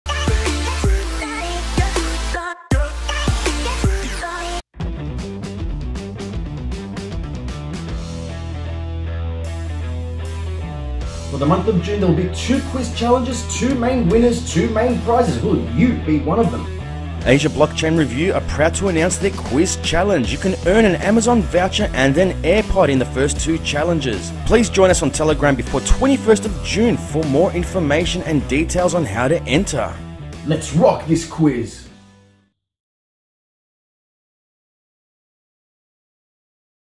Asia Blockchain Review
2 Quiz Challenges 2 winners and 2 prizes for the month of June! The quizes will be conducted in the Asia Blockchain Review Telegram group found this Friday 21/06 at 18:30 GMT +7 and will be open for several hours to the world, and the winner will be announced in the next episode by Darko!

This is just the begining with more quiz contests coming!